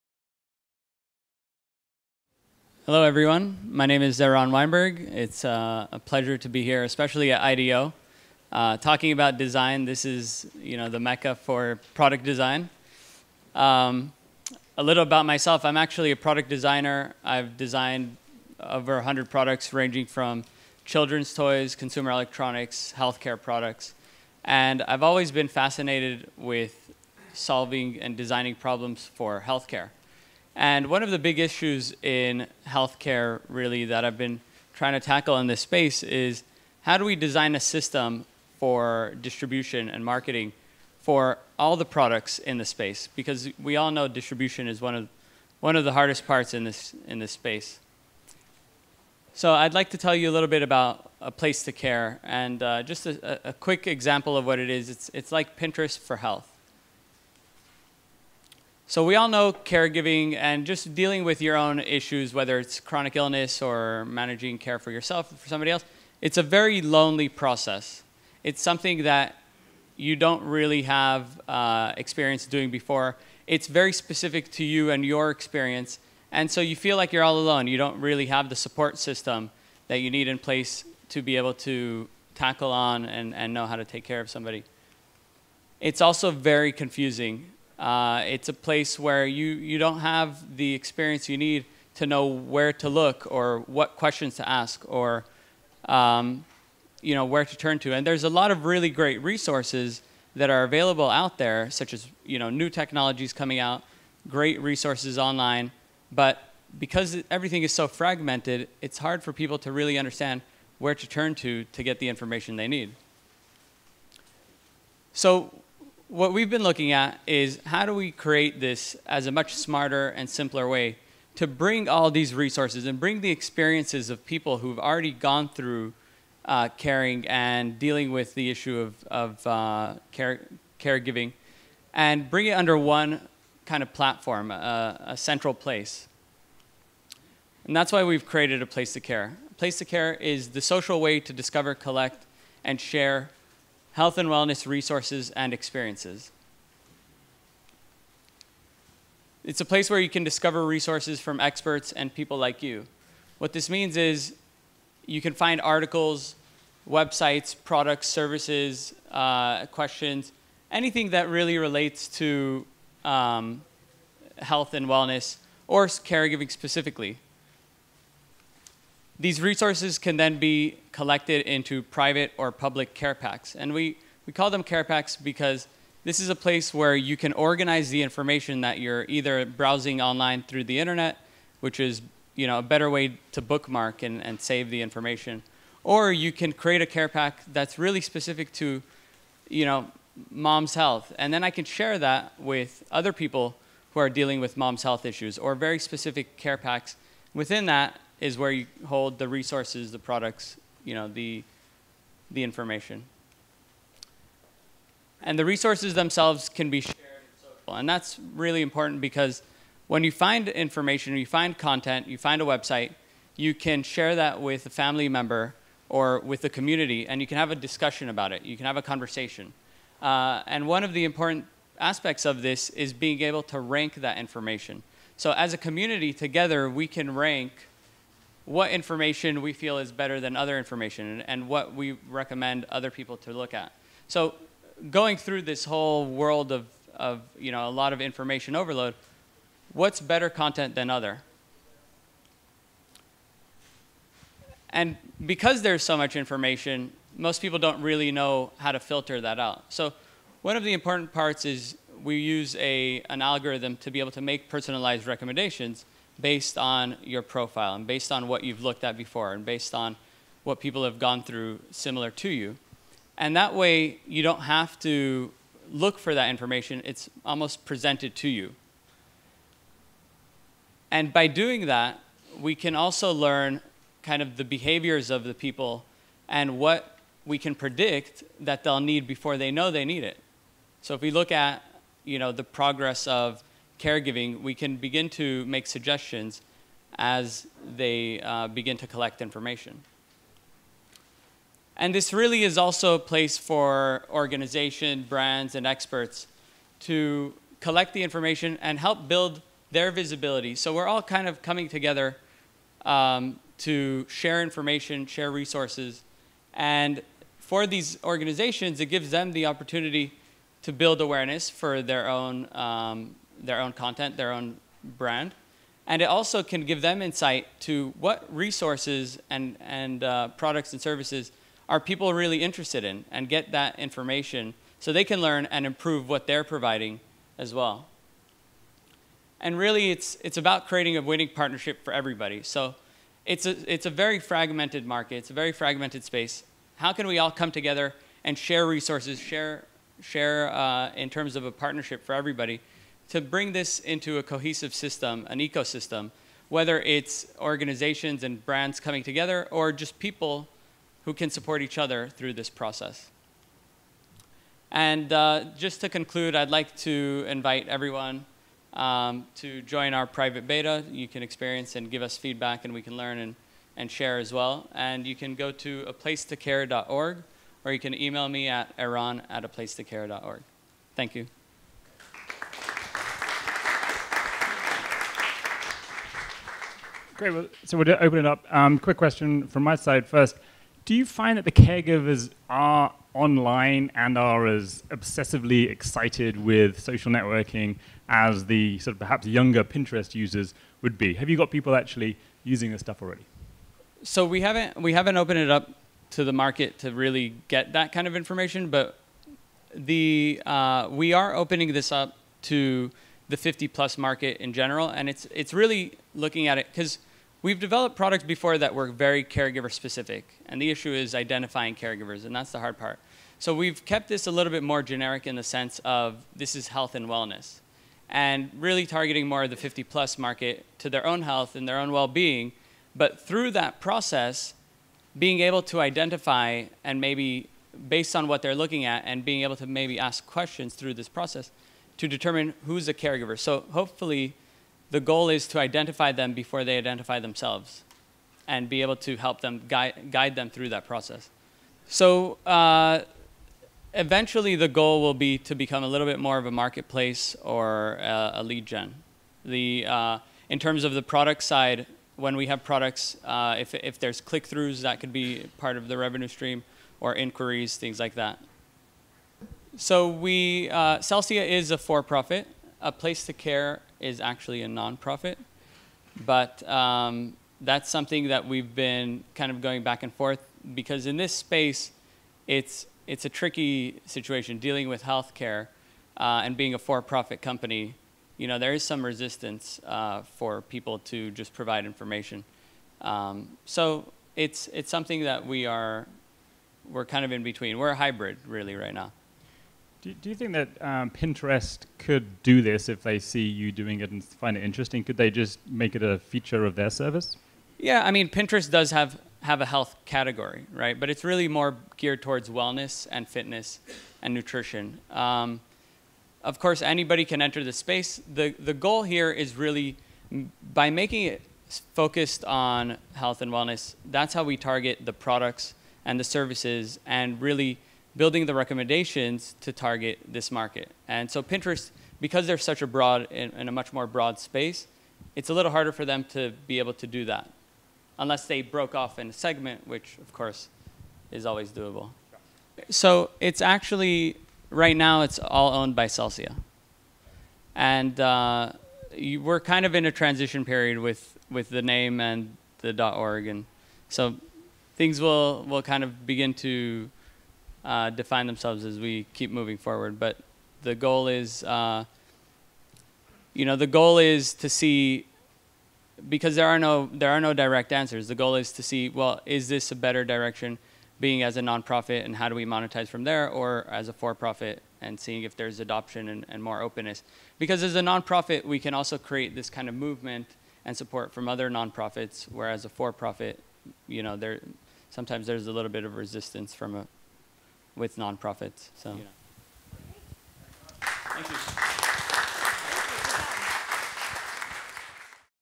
Eran Weinberg - Celsya @ Aging 2.0 | San Francisco, CA (05.23.2013)
Eran Weinberg is a designer, inventor, and entrepreneur who believes that through design and simple ideas we can improve peoples' lives. In 2005 while caring for his grandmother, Eran wanted a better solution to help her manage her daily life, stay independent and enable the family to support on her needs. Eran conceived of Celsya, an online and mobile solution, for which he received a 1st place international business plan award.  Celsya Inc designs and develops various tech-enabled health and wellness tools, many for caregiving. Product include Celsya Life, CareHub, eCare ID, CallGiver and Text-to-Track.

Eran is also the founder of Inclusive Design & Development, a value-added global design, development and consulting company. Earlier in his career, Eran worked as a senior designer for Cisco. Eran earned his degree in Industrial Engineering from Art Center College of Design.